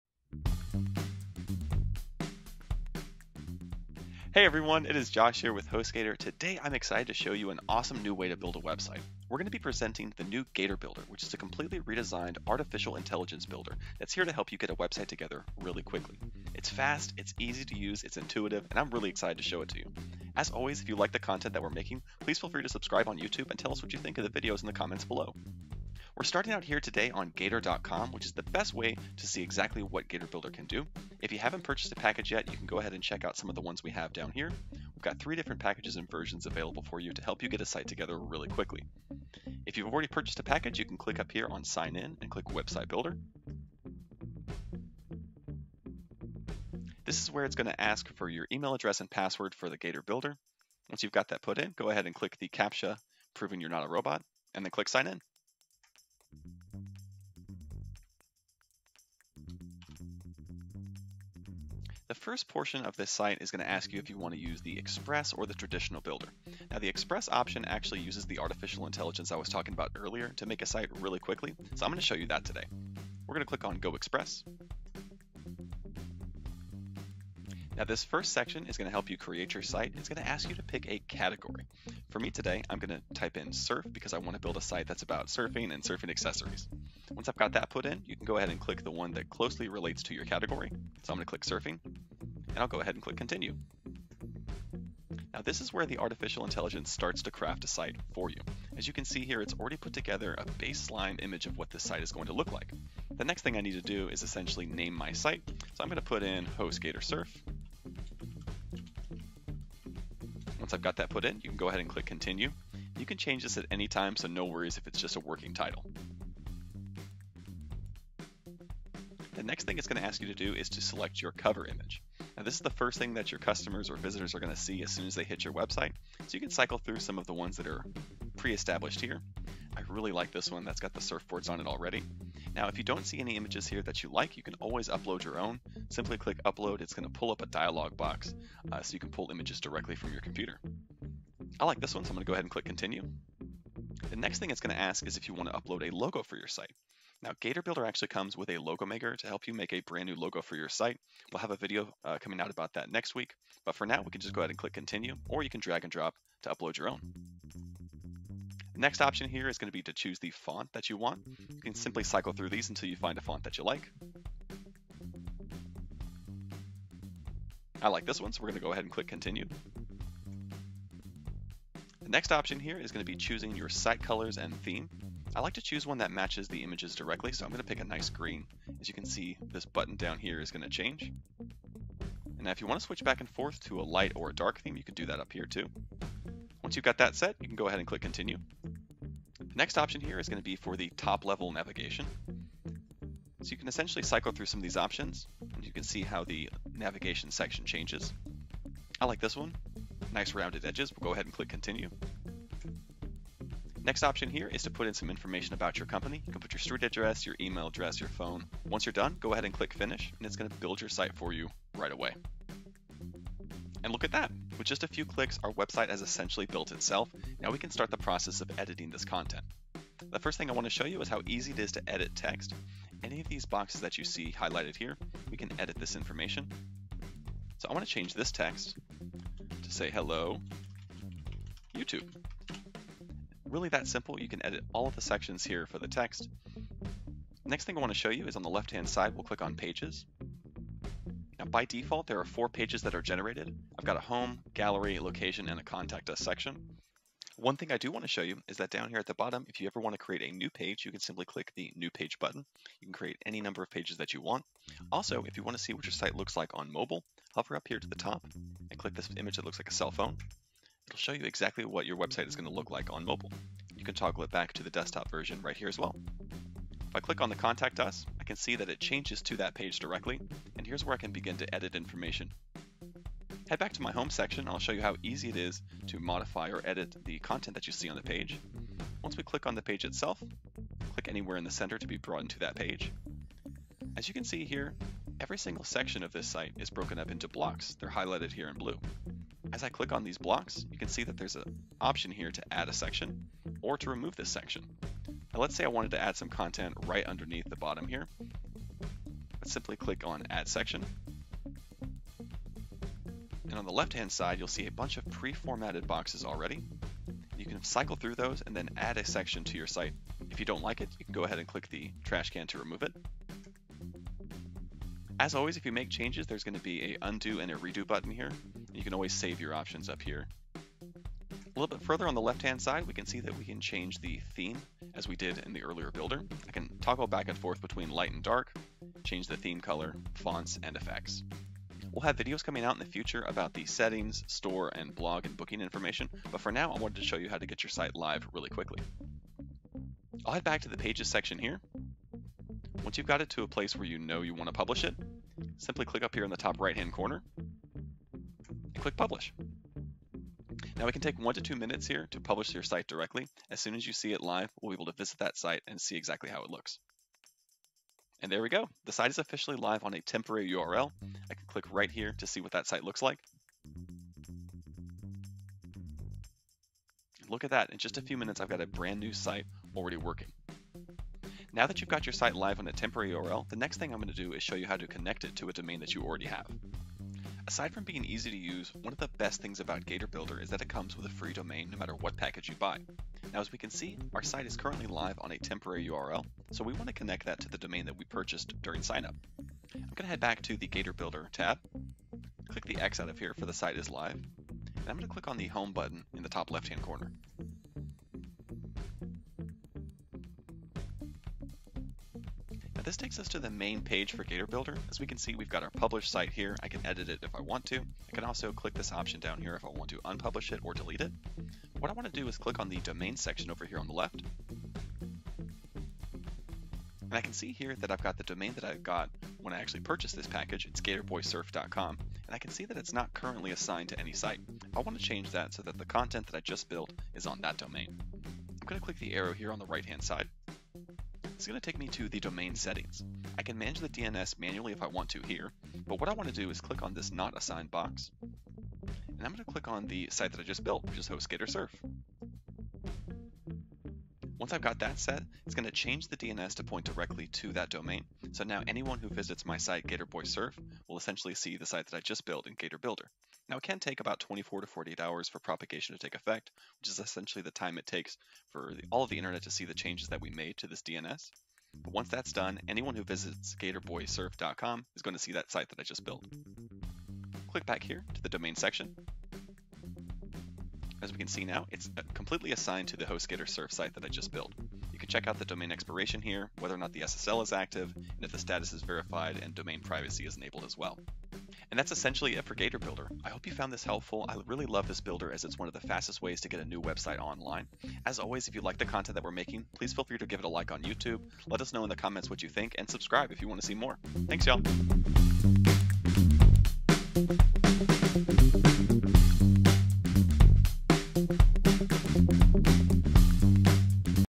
How to Use the New Gator Website Builder - HostGator Tutorial
Building a website doesn't need to be hard anymore with the Gator AI Website Builder. This new website builder tool from HostGator is going to make the process of building your website virtually effortless.

Thanks to its new AI Builder feature, it will do most of the building for you based on a few bits of information that you give it. If time is of the essence, but sacrificing performance is not an option, then Gator Website Builder is the choice for you.

Gator Builder Features various tools that will allow you to fully customize your site after the AI builder has finished constructing it. Follow along as Josh takes you through everything you will need to know to build your new website and make it uniquely yours.

In This video we will cover:

00:00 - Intro
00:40 - Signing in to Gator Builder
01:15 - Choosing a Category
01:40 - Naming your Website
01:58 - Customizing Themes and Layout
03:21 - Adding Contact Details
03:39 - Editing Text
03:55 - Managing Pages and Mobile View
04:34 - Block Editor and Adding Content
05:38 - Publishing your Website
06:13 - Temporary URL and Connecting your Domain